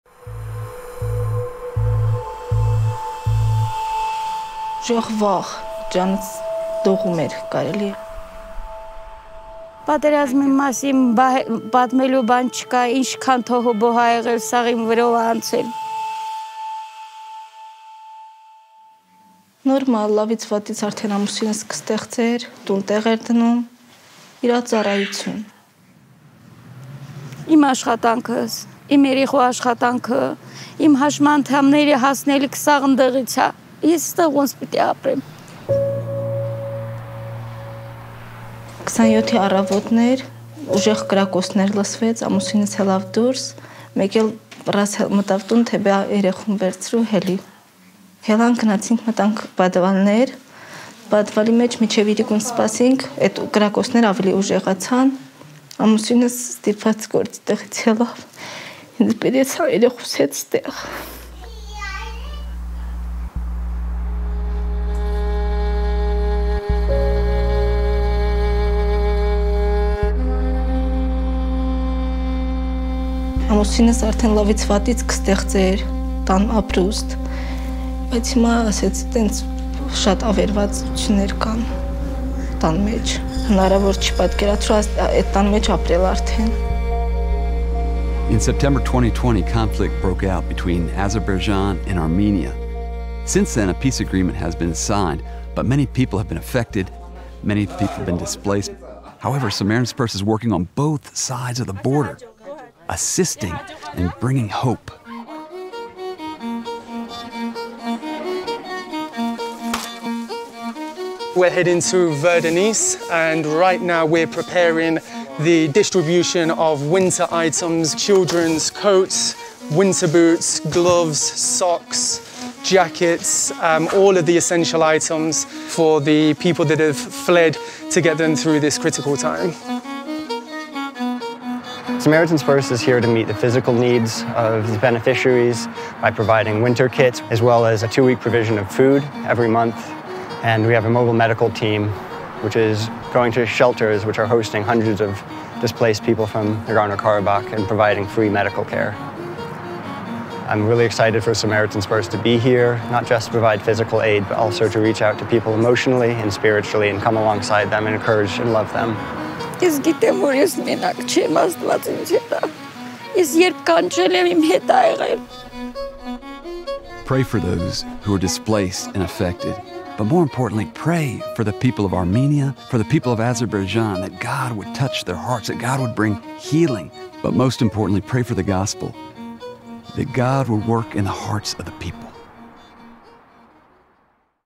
Preparing Families for Winter in Armenia
Samaritan’s Purse is in Armenia providing warm winter clothing, food, and medical care to families who are fleeing war and need to prepare for the oncoming winter.